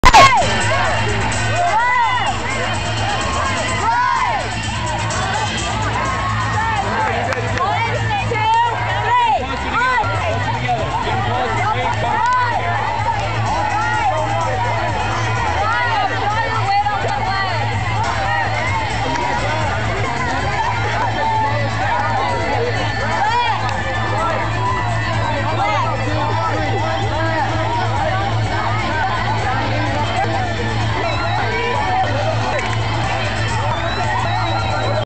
FC Camp Dry Creek, Louisiana :) 2008
Tournament Day...
The Skis. I am the third one back in the red shorts :)